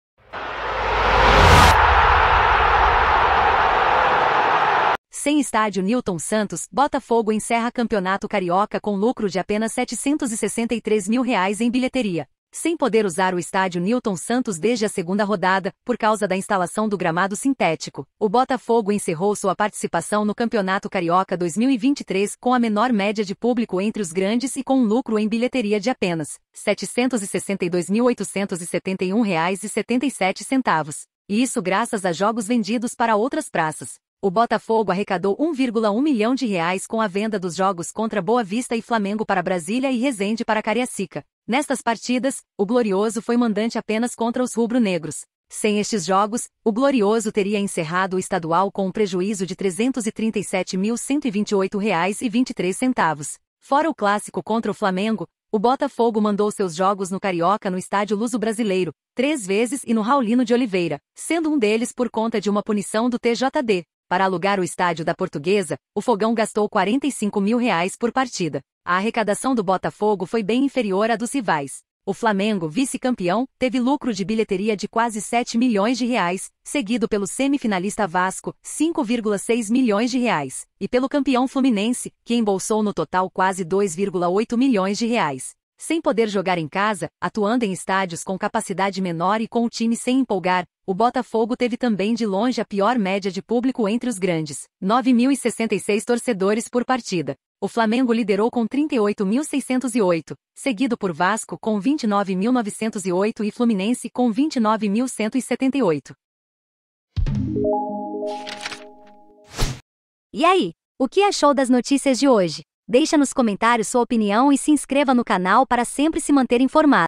CHEGOU A HORA DA TORCIDA ABRAÇAR O TIME? | CONFIRA
Sem Estádio Nilton Santos, Botafogo encerra Campeonato Carioca com lucro de apenas R$ 763 mil em bilheteria.
Sem poder usar o Estádio Nilton Santos desde a segunda rodada, por causa da instalação do gramado sintético, o Botafogo encerrou sua participação no Campeonato Carioca 2023 com a menor média de público entre os grandes e com um lucro em bilheteria de apenas R$ 762.871,77
e isso graças a jogos vendidos para outras praças.
O Botafogo arrecadou R$ 1,1 milhão com a venda dos jogos contra Boavista e Flamengo para Brasília e Resende para Cariacica. Nestas partidas, o Glorioso foi mandante apenas contra os rubro-negros. Sem estes jogos, o Glorioso teria encerrado o Estadual com um prejuízo de R$ 337.128,23.
Fora o clássico contra o Flamengo, o Botafogo mandou seus jogos no Carioca no Estádio Luso-Brasileiro, três vezes e no Raulino de Oliveira, sendo um deles por conta de uma punição do TJD. Para alugar o estádio da Portuguesa, o Fogão gastou R$ 45 mil por partida.
A arrecadação do Botafogo foi bem inferior à dos rivais. O Flamengo, vice-campeão, teve lucro de bilheteria de quase R$ 7 milhões, seguido pelo semifinalista Vasco (R$ 5,6 milhões) e pelo campeão Fluminense, que embolsou no total quase R$ 2,8 milhões.
Sem poder jogar em casa, atuando em estádios com capacidade menor e com o time sem empolgar, o Botafogo teve também de longe a pior média de público entre os grandes: 9.066 torcedores por partida. O Flamengo liderou COM 38.608, seguido por Vasco COM 29.908 e Fluminense COM 29.178